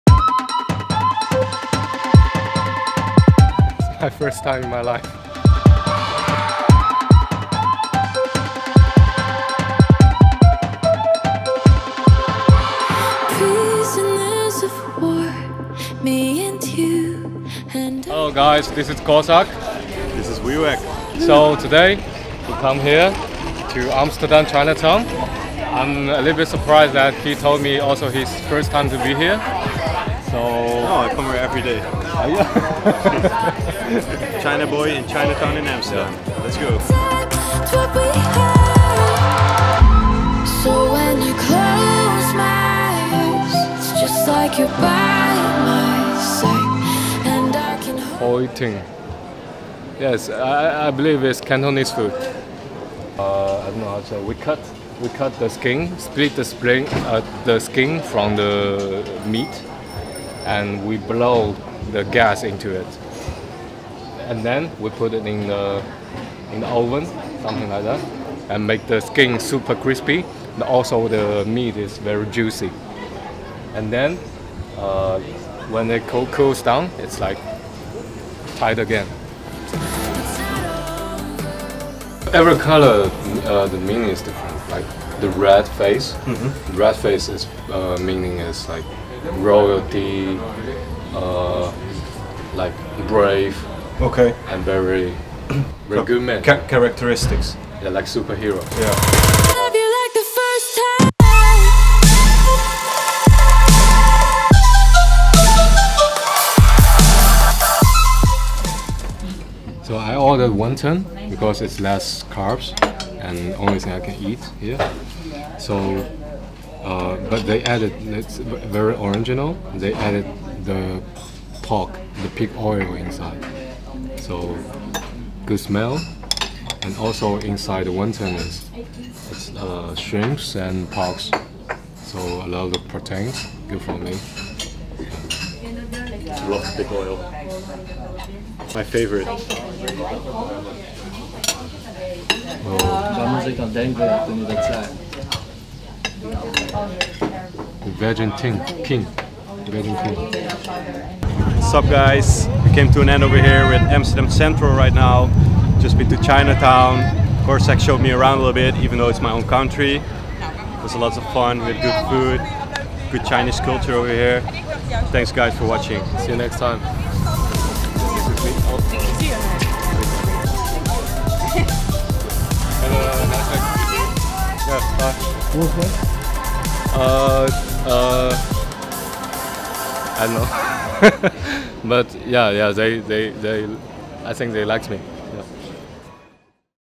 Chinatown Around the World - Amsterdam
Wiwek and I went around Amsterdam this July before my show at Tomorrowland to check out what their Chinatown had to offer. Loved being back in Holland where I first travelled to four years ago.

Where should I go next?